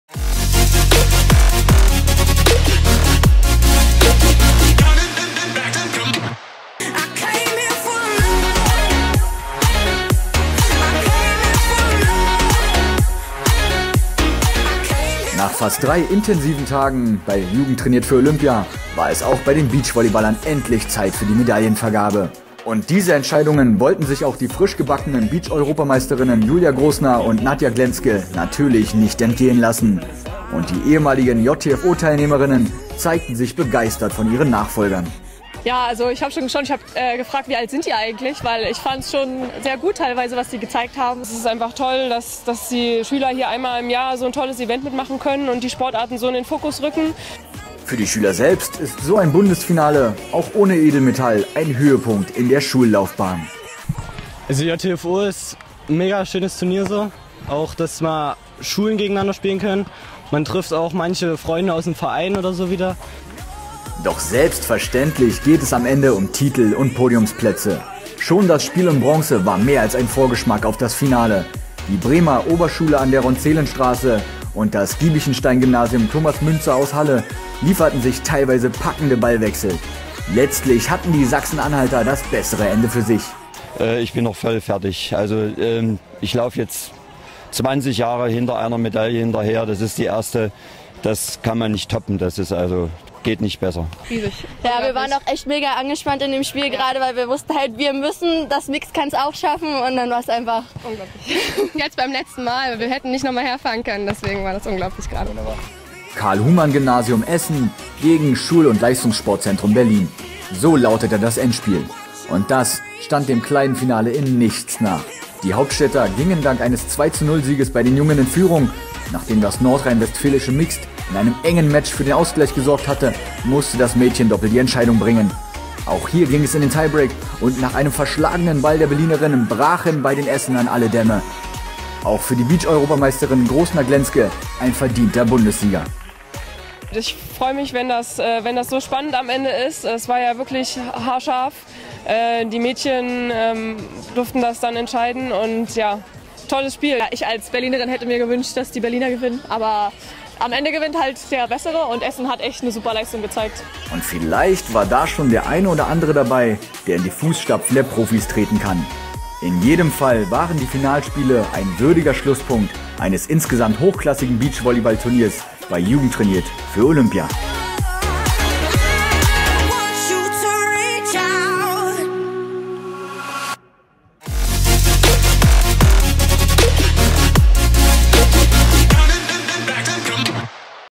JTFO / JTFP | Herbstfinale 2017 Tag 3 - Finale Beachvolleyball
Nach fast drei intensiven Tagen bei Jugend trainiert für Olympia war es auch bei den Beachvolleyballern endlich Zeit für die Medaillenvergabe. Und die Finalspiele waren ein würdiger Schlusspunkt eines insgesamt hochklassigen Turniers.